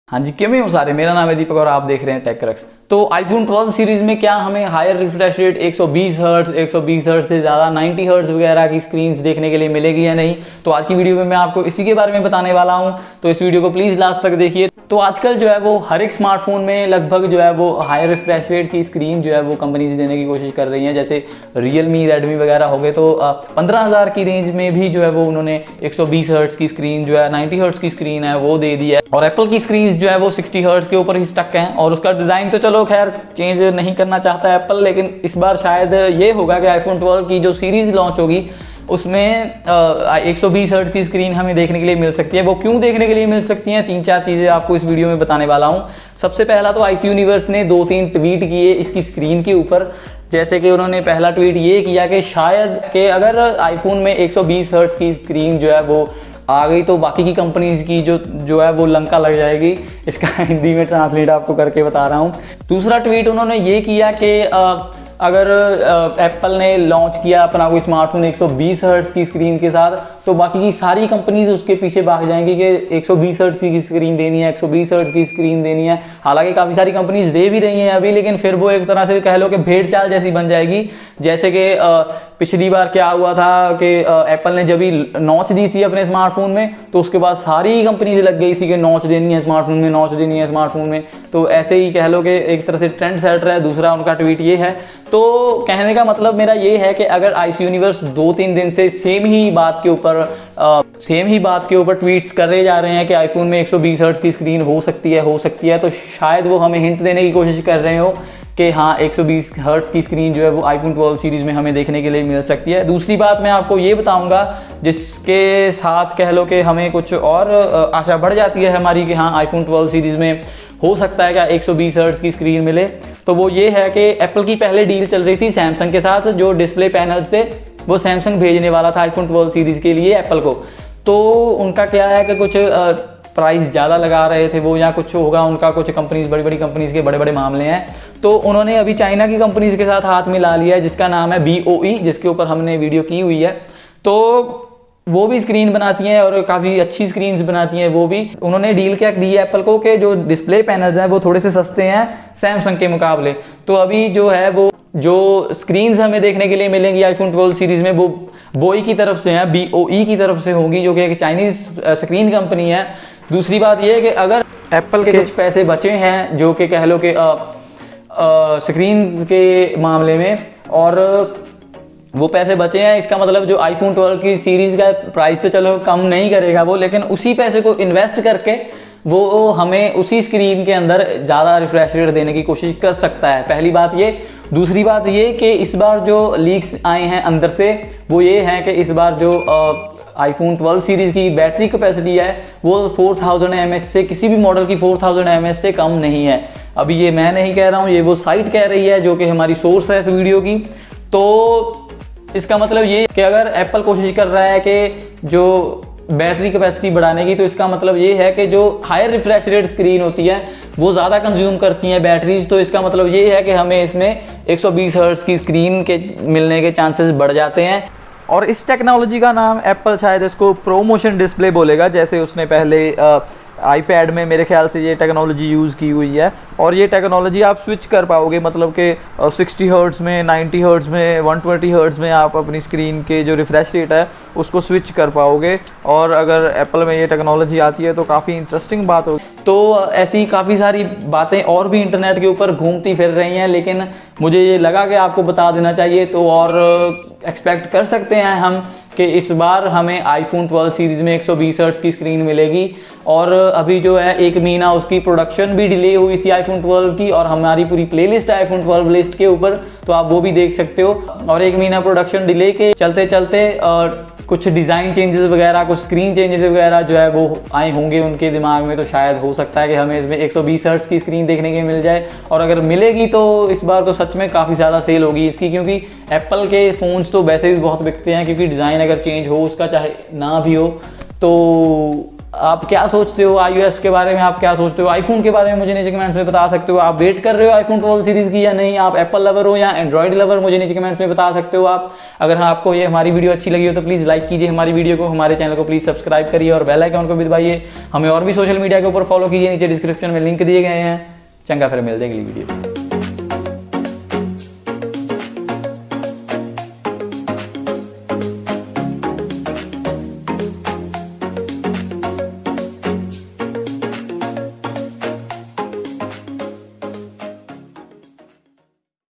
Iphone 12 Series - 120 Hz Screen On All Models ?||TECHCRUX
Hello guys ! Iss video mein maine aapse iphone 12 Series mein aane wale naye feature ke baare mein baat ki hai.iphone 12 series mein apple 120 Hz Ki screen dene ki soch raha hai. Aur jaankaari ke liye video ko poora dekhe.Agar video achi lage to plz like kre aur share kre.
My gears!!!